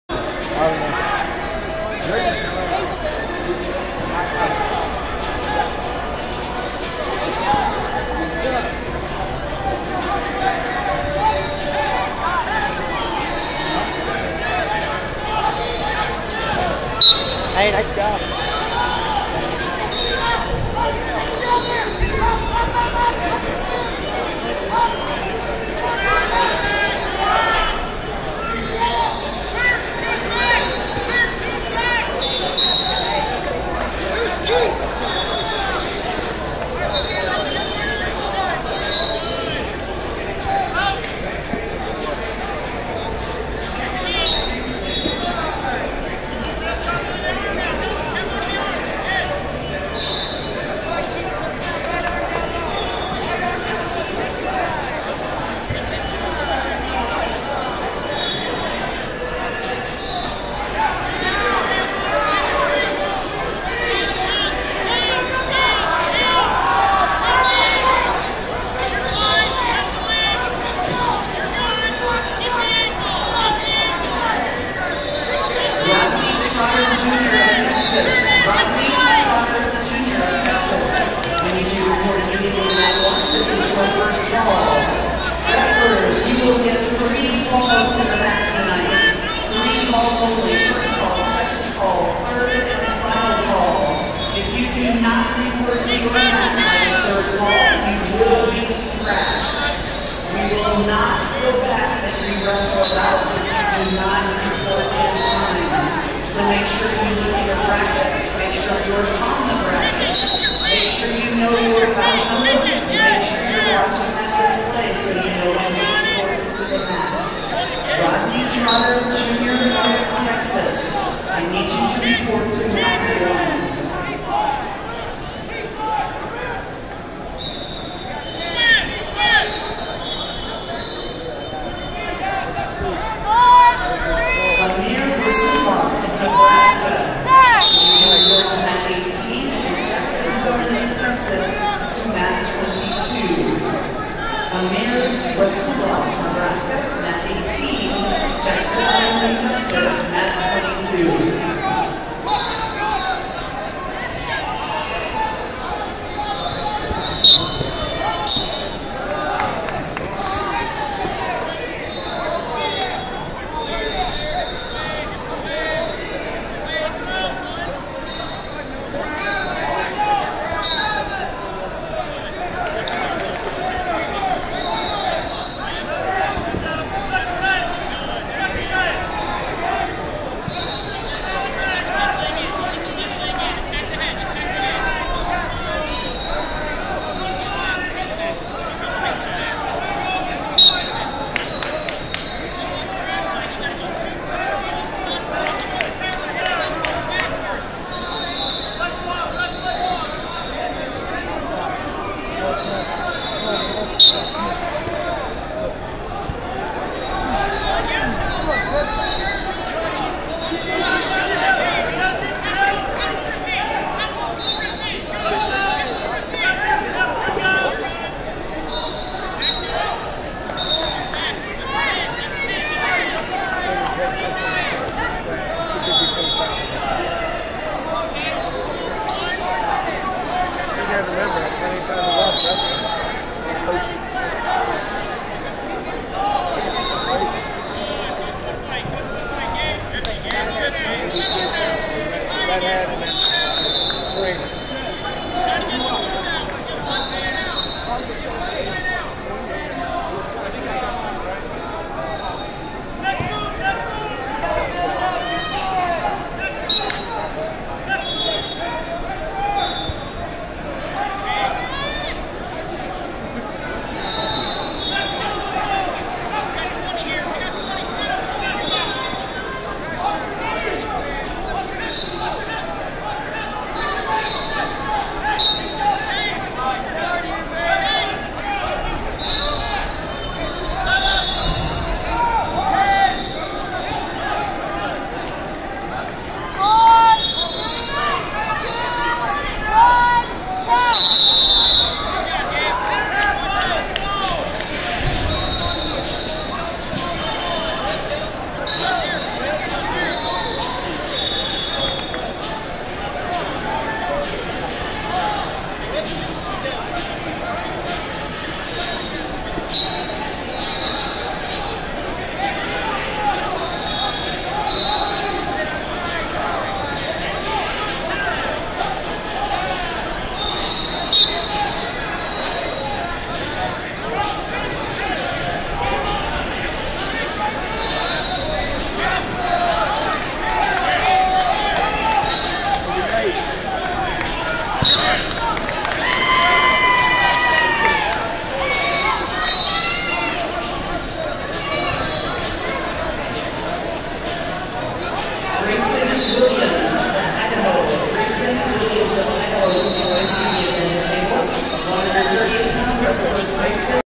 132 Lbs Champ. Round 1 - Vance Barz, MN Vs Gabe Turman, NE 2db0
Flosports-partner-access/2022 Brian Keck Memorial Preseason Nationals-Recruiting Showcase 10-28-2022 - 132 Lbs Champ. Round 1 - Vance Barz, MN Vs Gabe Turman, NE 2db0